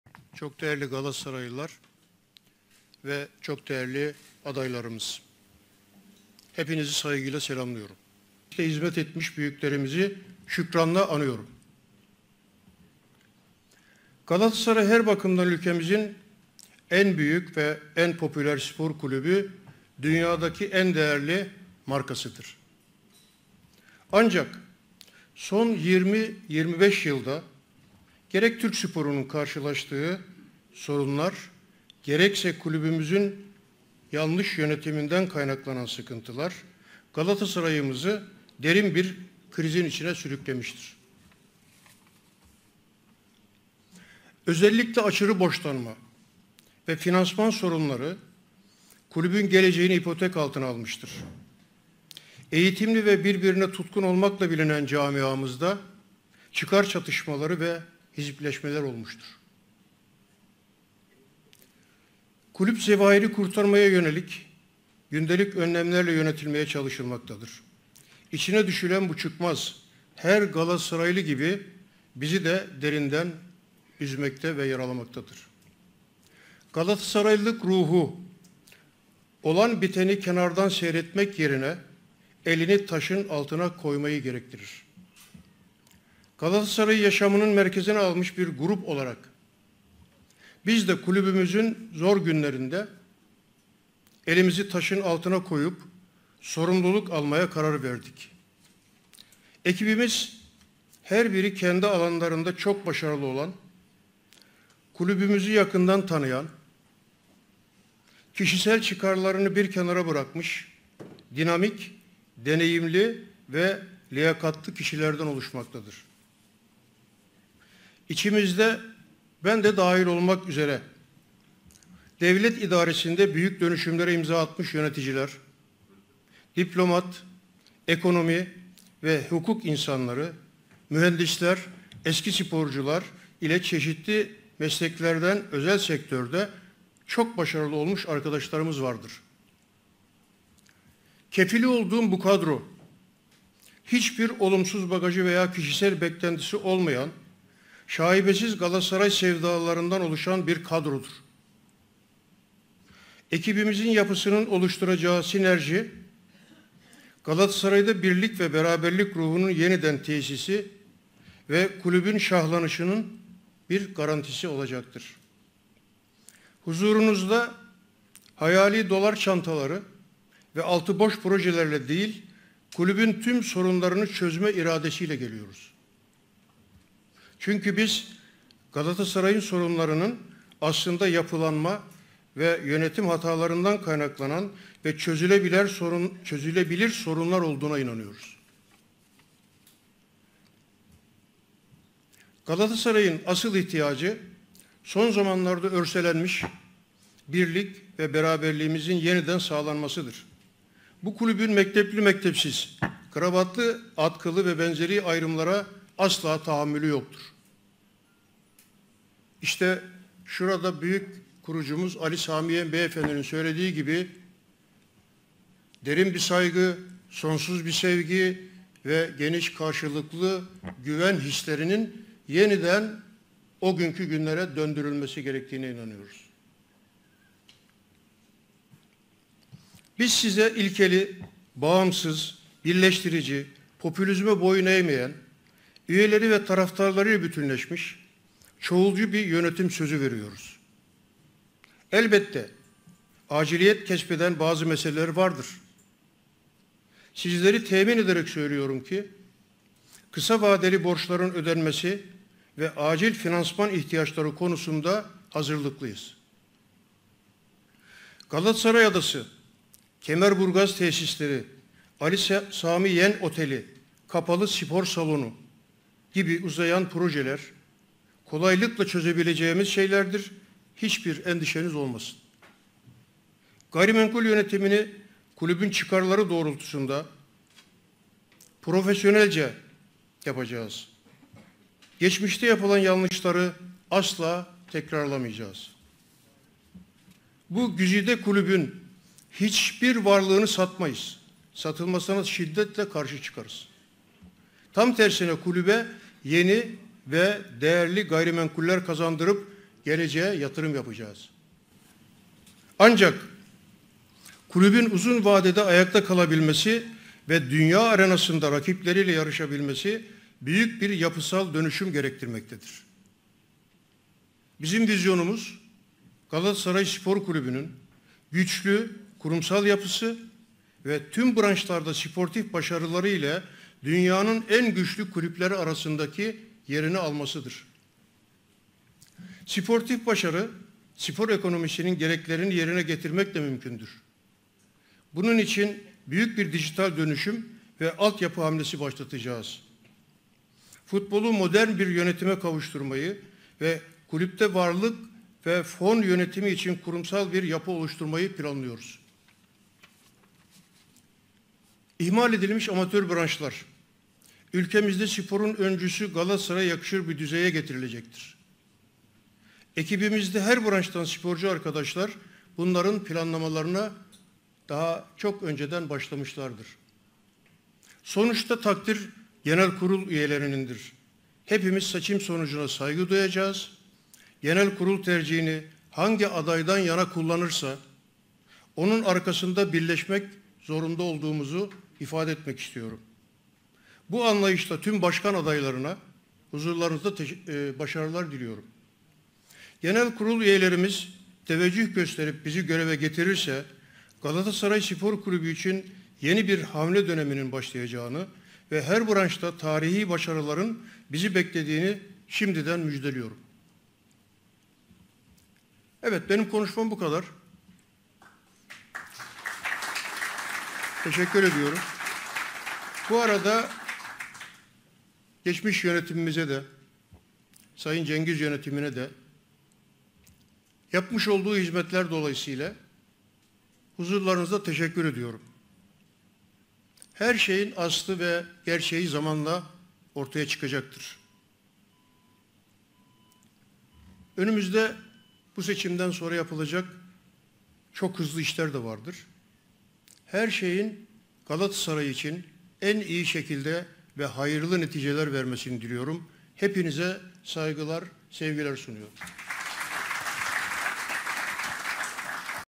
İbrahim Özdemir: Bu güzide kulübün hiçbir varlığını satmayız! | Galatasaray
Galatasaray Başkan Adayı İbrahim Özdemir, kulübün tüm sorunlarını çözme iradesiyle geldiklerini söyledi.
Galatasaray Başkan Adayı İbrahim Özdemir, divan kurulu toplantısında konuştu. Galatasaray’ın Türkiye’nin dünyadaki en değerli markası olduğunu belirten Özdemir, "Ancak son 20-25 yılda gerek Türk sporunu karşılaştığı sorunlar gerekse kulübümüzün yönetiminden kaynaklanan sıkıntılar, Galatasarayımızı derin bir krizin içine sürüklemiştir. Özellikle aşırı borçlanma ve finansman sorunları, kulübün geleceğini ipotek altına almıştır. Kulübü kurtarmaya yönelik gündelik önlemlerle yönetilmeye çalışılmaktadır. İçine düşülen bu çıkmaz, her Galatasaraylı gibi bizi de derinden üzmekte ve yaralamaktadır. Galatasaraylılık ruhu olan biteni kenardan seyretmek yerine elini taşın altına koymayı gerektirir. Galatasaray’ı yaşamının merkezine almış bir grup olarak biz de kulübümüzün zor günlerinde elimizi taşın altına koyup sorumluluk almaya karar verdik. Ekibimiz her biri kendi alanlarında çok başarılı olan, dinamik, deneyimli ve liyakatlı kişilerden oluşmaktadır. Kefil olduğum bu kadro, kişisel beklentisi olmayan, şaibesiz Galatasaray sevdalılarından oluşan bir kadrodur. Ekibimizin oluşturacağı sinerji, Galatasaray’da birlik ve beraberlik ruhunu yeniden tesisi ve kulübün şahlanışının bir garantisi olacaktır. Hayali dolar çantaları ve altı boş projelerle değil, kulübün tüm sorunlarını çözme iradesiyle geliyoruz. Çünkü biz Galatasaray’ın sorunlarının aslında yapılanma ve yönetim hatalarından kaynaklanan ve çözülebilir sorunlar olduğuna inanıyoruz. Galatasaray’ın asıl ihtiyacı, son zamanlarda örselenmiş birlik ve beraberliğimizin yeniden sağlanmasıdır. Biz size ilkeli, bağımsız, birleştirici, popülizme boyun eğmeyen, üyeleri ve taraftarlarıyla bütünleşmiş çoğulcu bir yönetim sözü veriyoruz. Sizleri temin ederek söylüyorum ki kısa vadeli borçların ödenmesi ve acil finansman ihtiyaçları konusunda hazırlıklıyız. Galatasaray Adası, Kemerburgaz Tesisleri, Ali Sami Yen Oteli, kapalı spor salonu gibi projeler, kolaylıkla çözebileceğimiz şeylerdir. Hiçbir endişeniz olmasın. Gayrimenkul yönetimini kulübün çıkarları doğrultusunda profesyonelce yapacağız. Geçmişte yapılan yanlışları asla tekrarlamayacağız. Bu güzide kulübün hiçbir varlığını satmayız. Satılmasına şiddetle karşı çıkarız. Tam tersine kulübe yeni ve değerli gayrimenkuller kazandırıp geleceğe yatırım yapacağız. Futbolu modern bir yönetime kavuşturmayı ve kulüpte varlık ve fon yönetimi için kurumsal bir yapı oluşturmayı planlıyoruz. İhmal edilmiş amatör branşlar, ülkemizde sporun öncüsü Galatasaray’a yakışır bir düzeye getirilecektir. Takdir genel kurul üyelerinindir. Hepimiz seçim sonucuna saygı duyacağız. Genel kurul tercihini hangi adaydan yana kullanırsa onun arkasında birleşmek zorunda olduğumuzu ifade etmek istiyorum. Bu anlayışla tüm başkan adaylarına başarılar diliyorum" ifadelerini kullandı.